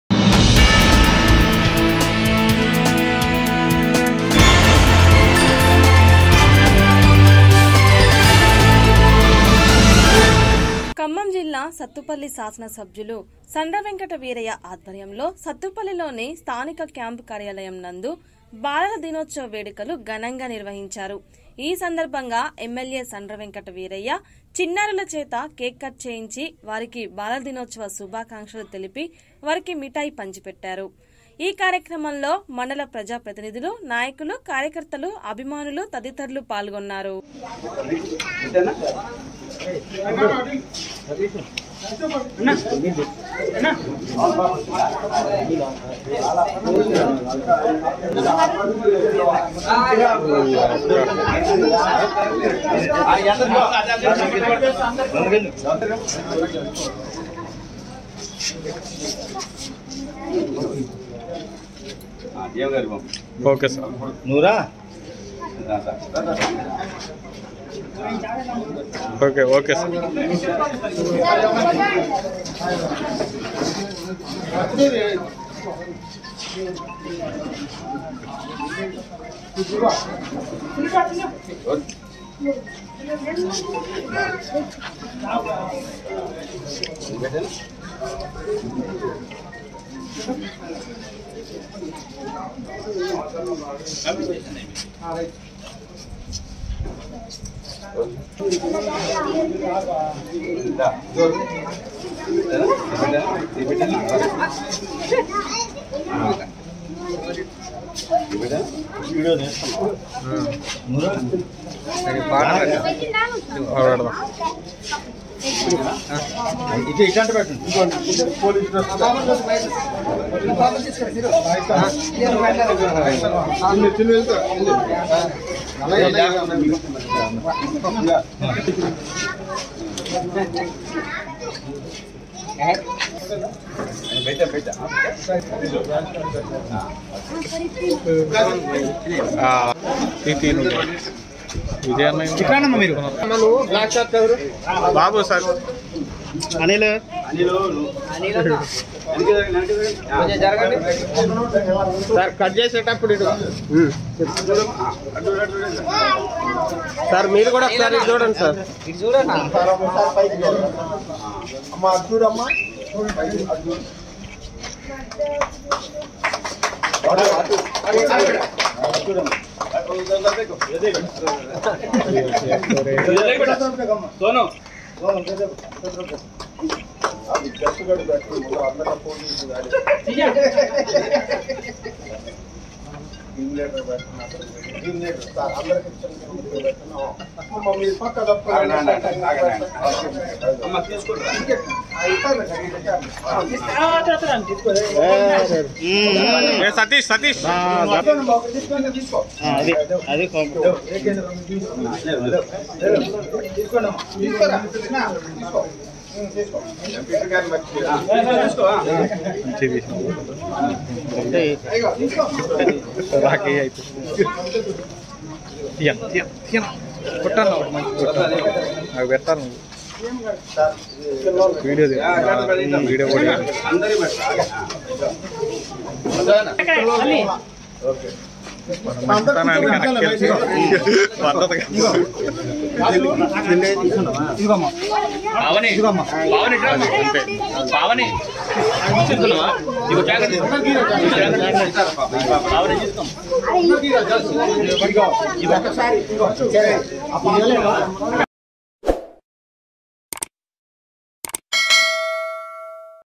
ఎమ్మెల్యే సండ్ర వెంకట వీరయ్య ఆధ్వర్యంలో బాలల దినోత్సవ వేడుకలు | DNB NEWS
ఘనంగా ఎమ్మెల్యే సండ్ర వెంకట వీరయ్య  ఆధ్వర్యంలో క్యాంప్ కార్యాలయం నందు బాలల దినోత్సవ వేడుకలు.

సత్తుపల్లి శాసనసభ్యులు సండ్ర వెంకట వీరయ్య  ఆధ్వర్యంలో సత్తుపల్లిలోని స్థానిక క్యాంప్ కార్యాలయం నందు బాలల దినోత్సవ వేడుకలు ఘనంగా నిర్వహించారు... ఈ

సందర్భంగా ఎమ్మెల్యే సండ్ర వెంకట వీరయ్య  చిన్నారుల చేత కేక్ కట్ చేయించి వారికి బాలల దినోత్సవ శుభాకాంక్షలు తెలిపి వారికి మిఠాయిలు పంచారు. ఈ కార్యక్రమంలో మండల ప్రజా ప్రతినిధులు నాయకులు, కార్యకర్తలు అభిమానులు తదితరులు పాల్గొన్నారు.